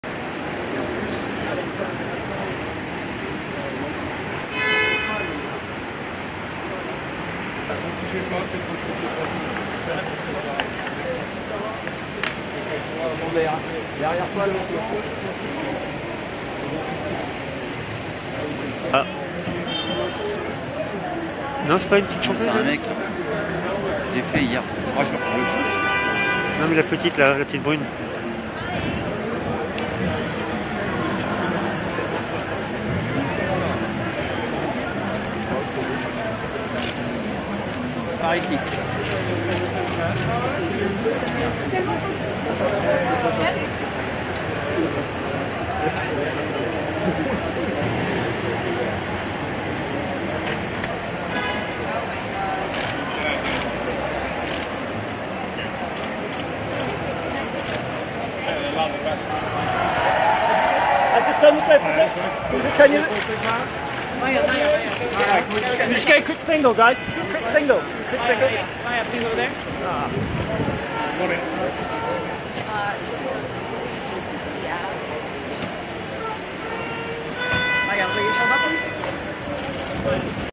Maya Henry, Christine and the Queens and more at the Vogue anniversary in Paris
We spotted Miss Fame, Maya Henry, Christine and the Queens and more at the Vogue Paris exhibition celebrating its 100th anniversary, at the Palais Galliera during the Paris Fashion Week

Friday 1, October 2021 - Paris, France